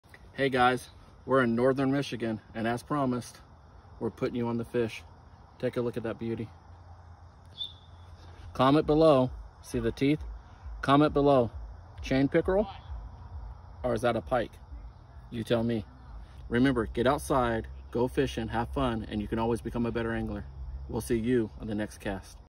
WHAT Did I CATCH? | Can YOU Tell The DIFFERENCE?
In this shorts video I am panfishing when I HOOK Into an unexpected CATCH! Is it a Pike or a Chain Pickerel?